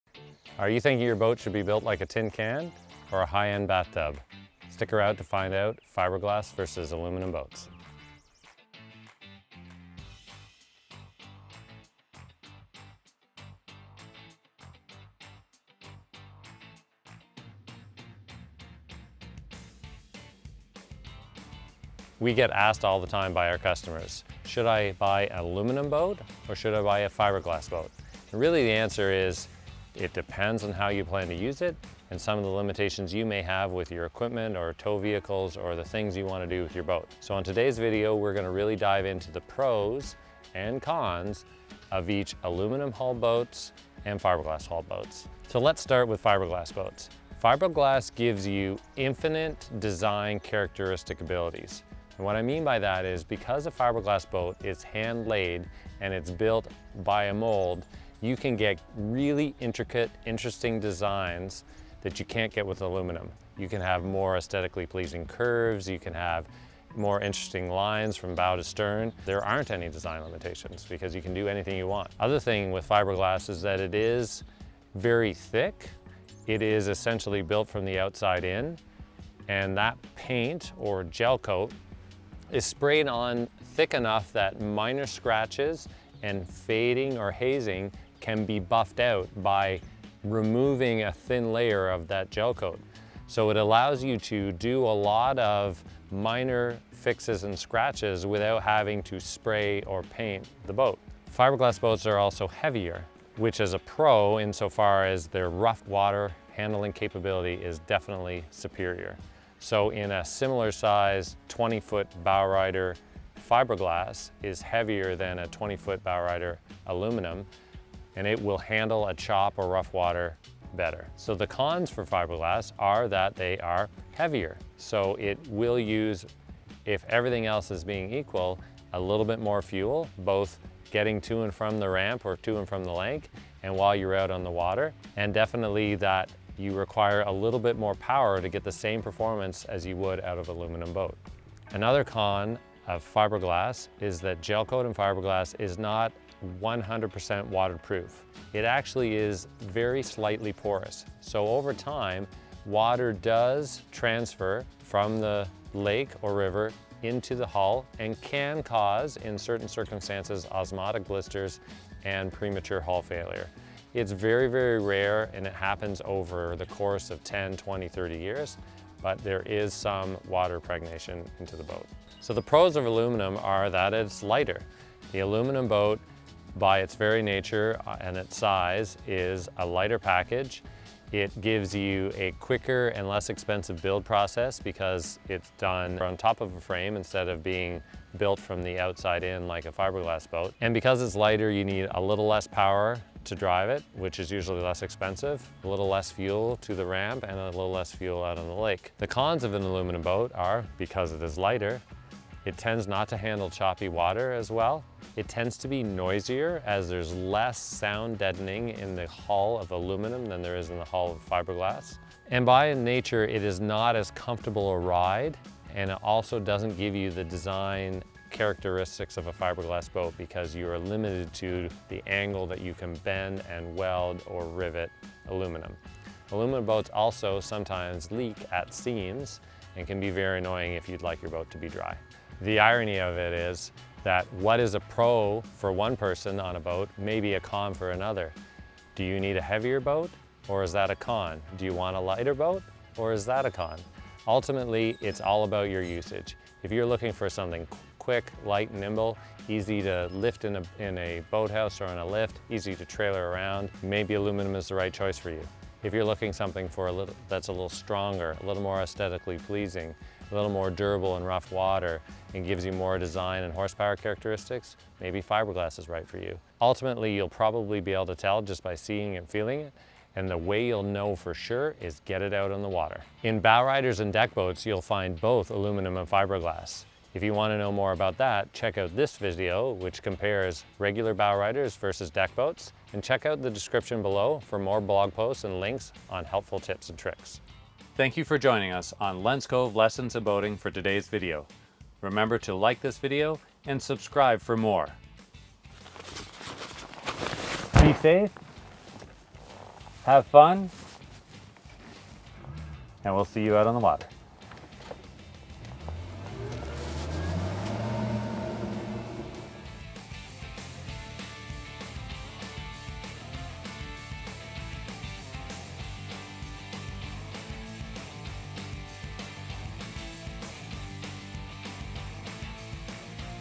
Aluminum vs Fiberglass Boats
What type of boat is better for you, aluminum or fiberglass? Choosing the size and style of boat is one thing, but what about the different material used to build them. Is one better than the other when you talk about aluminum vs Fiberglass? In this video we will cover:

1. Fiberglass Boats Pros and Cons. 0:46
2. Aluminum Boats Pros and Cons. 2:52
3. Recap 3:50